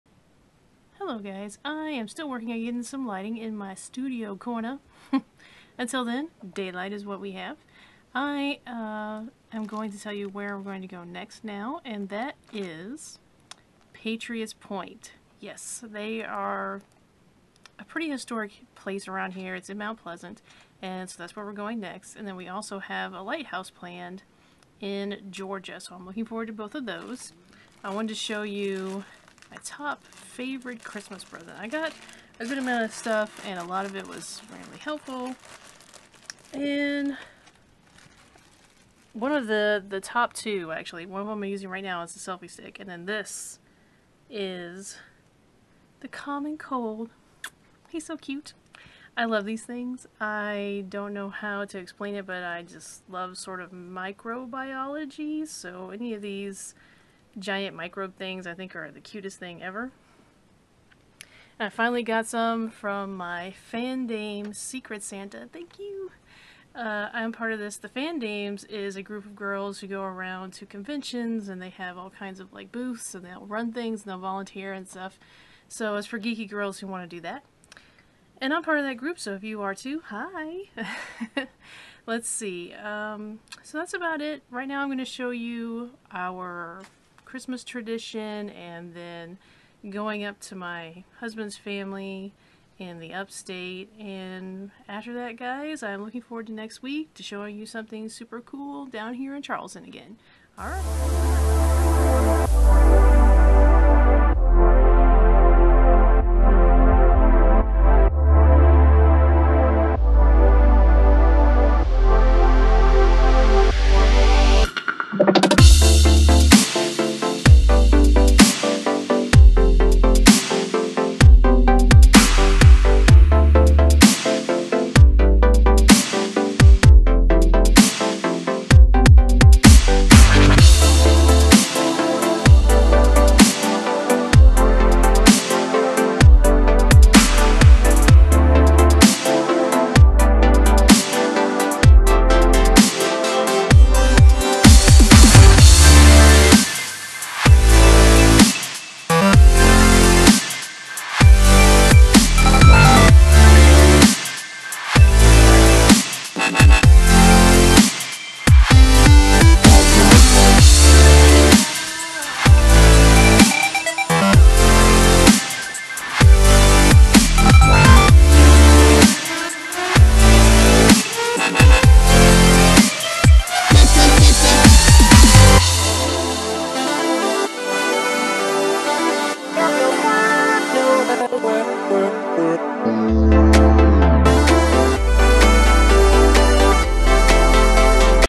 Vlog #18 | Belmond Place Charleston, Waterfall in Simpsonville, Lighthouses
2016 was a year full of wanders that I will always remember.  It's always good to get out of your door and down the road.  In this video I show you our Christmas round up with traditions, my favorite presents, and a wander up to Simpsonville to visit family.  I hope this caravan continues to grow as 2017 flies by!